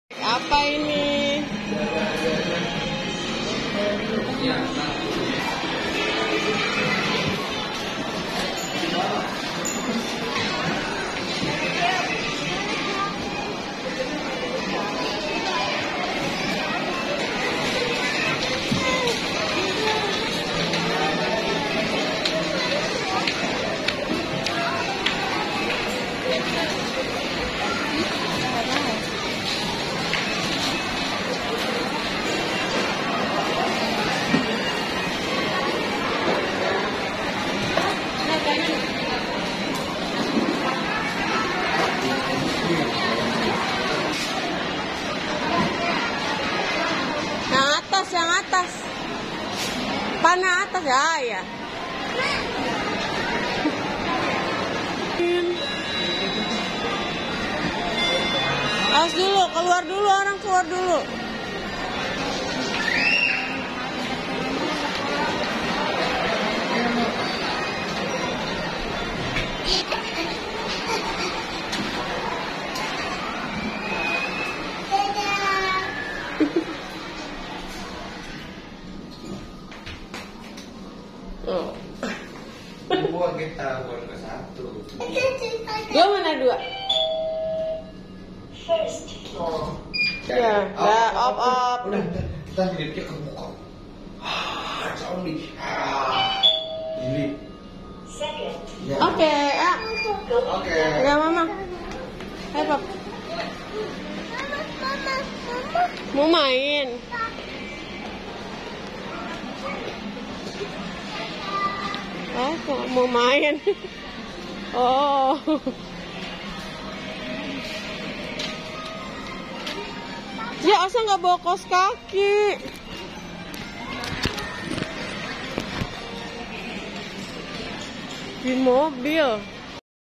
rencananya mau main tapi gak, so sad banget anak mami.. nanti ya kita main
@Uda_arshaVLOG  adalah baby viral tahun 2020 guys.. tentang kuning telor mentah